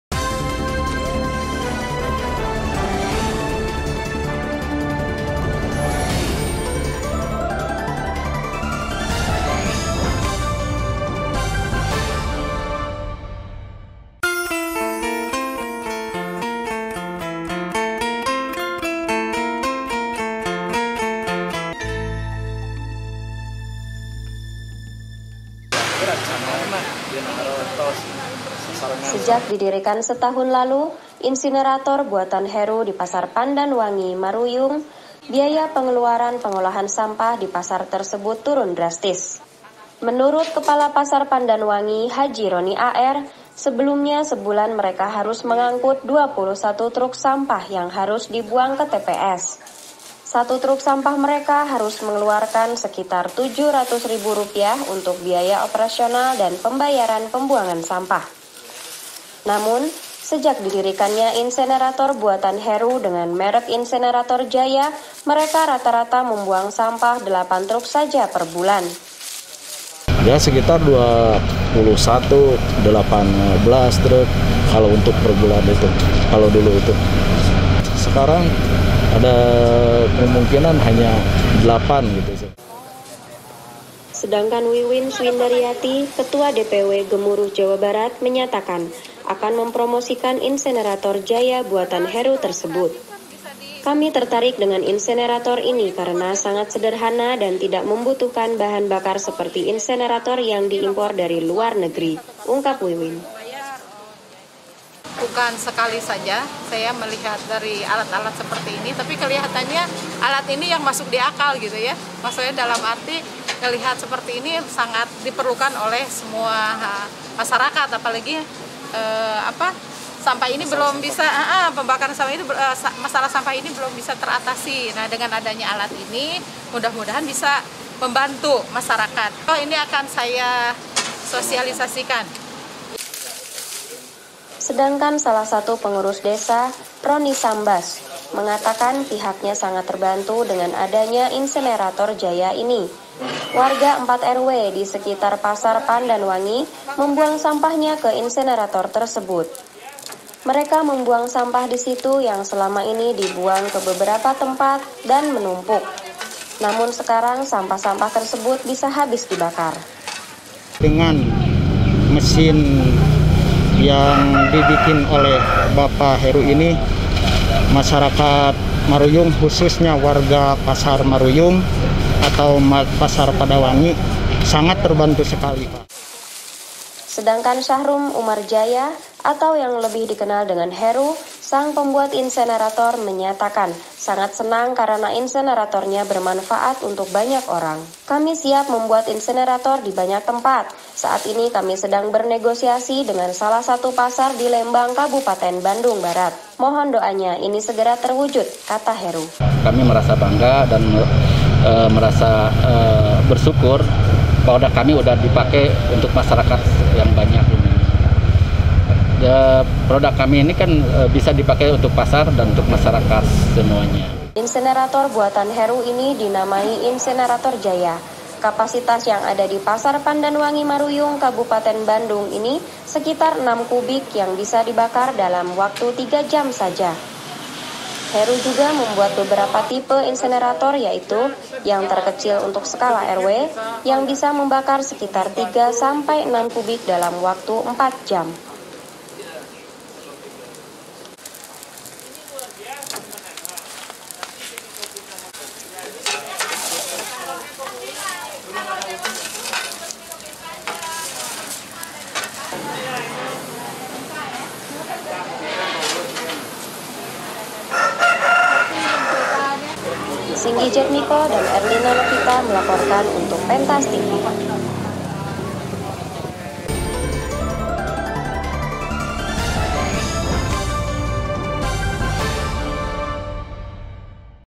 PENTAS TV - INSINERATOR JAYA BISA TURUNKAN BIAYA PEMBUANGAN SAMPAH DI PASAR
SEJAK DIDIRIKAN SETAHUN LALU,  INSENERATOR BUATAN HERU DI PASAR PANDAN WANGI, MARUYUNG,  BIAYA PENGELUARAN PENGOLAHAN SAMPAH DI PASAR TERSEBUT TURUN DRASTIS
MENURUT KEPALA PASAR PANDANWANGI, HAJI RONI A.R SEBELUMNYA SEBULAN MEREKA HARUS MENGANGKUT 21 TRUK SAMPAH YANG HARUS DIBUANG KE TPS.
SATU TRUK SAMPAH MEREKA HARUS MENGELUARKAN SEKITAR RP.  700.000 UNTUK BIAYA OPERASIONAL DAN PEMBAYARAN PEMBUANGAN SAMPAH NAMUN SEJAK DIDIRIKANNYA INSINERATOR BUATAN HERU DENGAN MERK INSINERATOR JAYA MEREKA RATA-RATA MEMBUANG SAMPAH 8 TRUK SAJA PERBULAN.